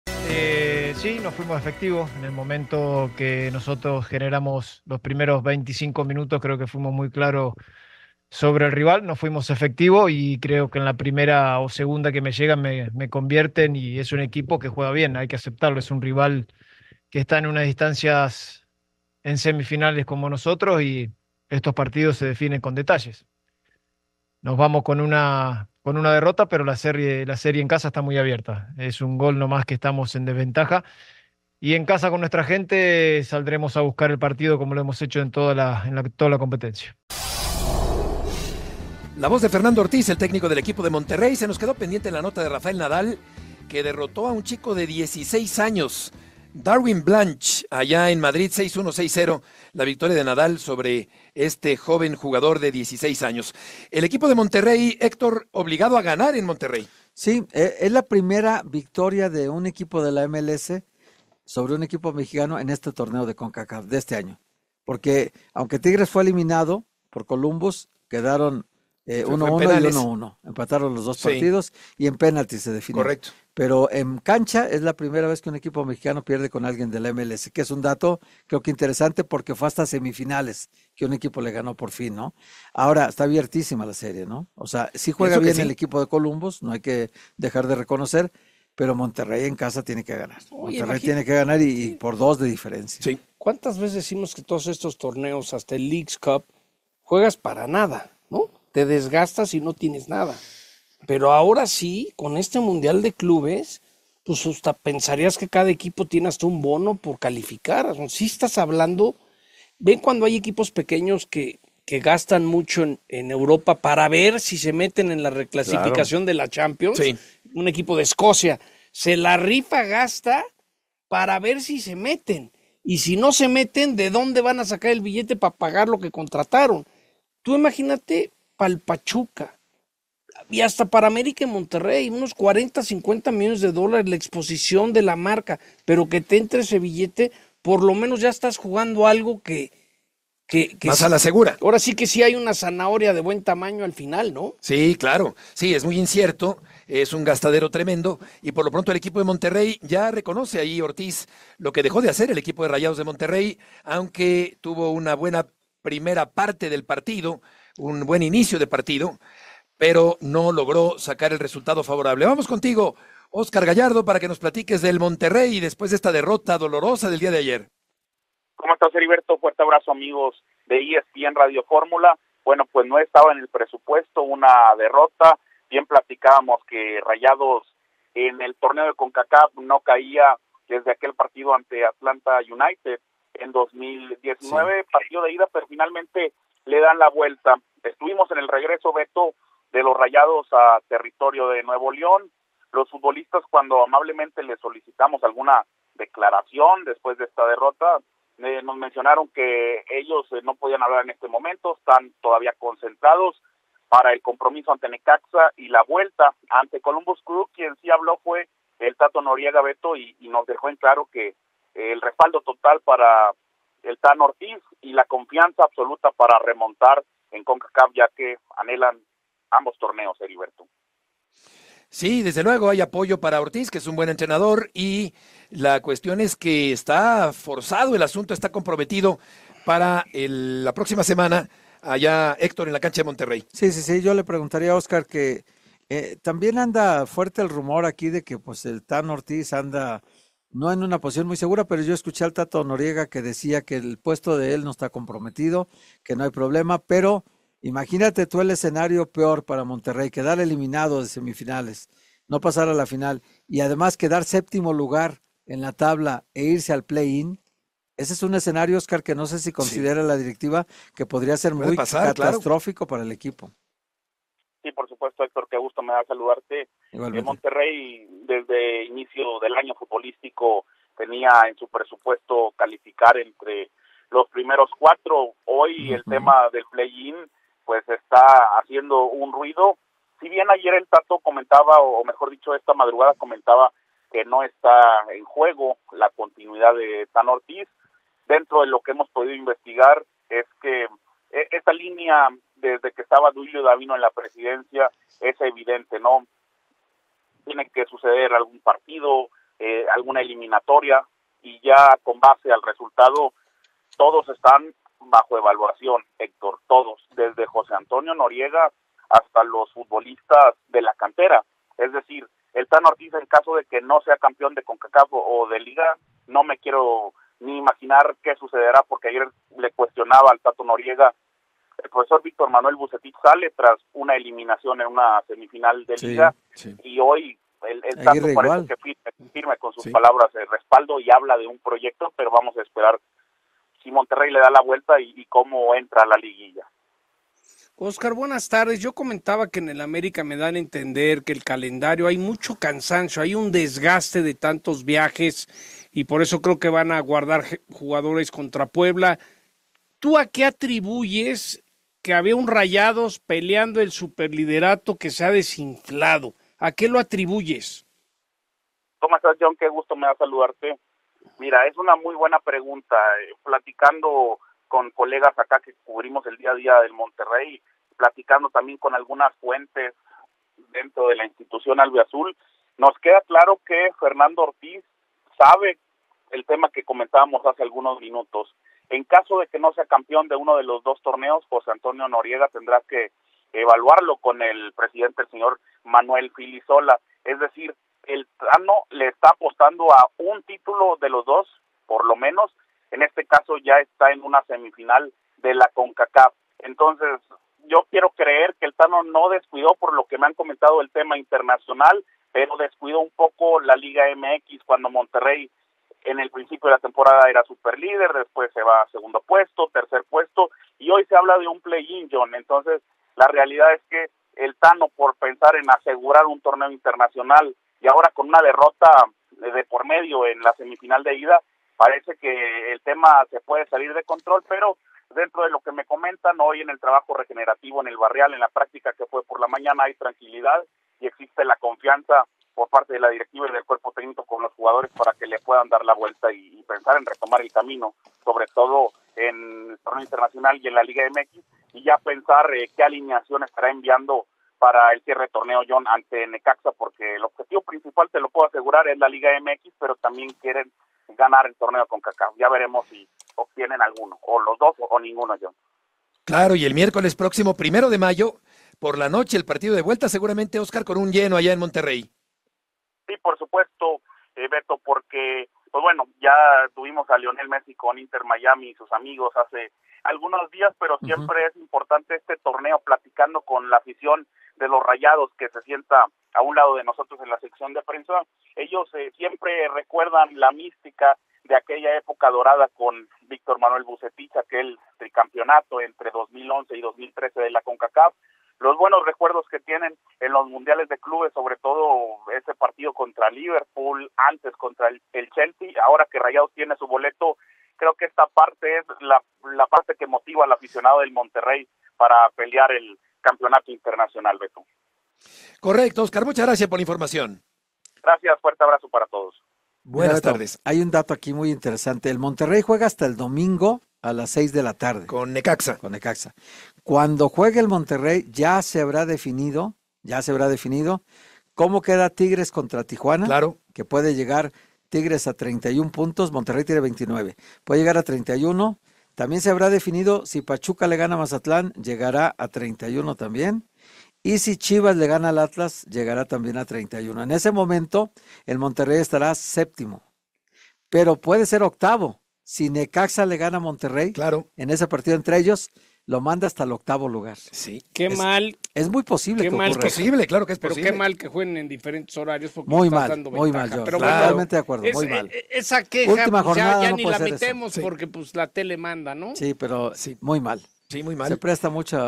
Rayados en la cuerda floja tras derrota ante Columbus
Luego de la derrota de Monterrey ante el Columbus Crew, Fernando ‘Tano’ Ortíz dijo que es un pequeño tropiezo pero que en la vuelta irán con todo para darle la vuelta al marcador, aunque deben ganar por dos goles. La mesa de espe

Grupo Fórmula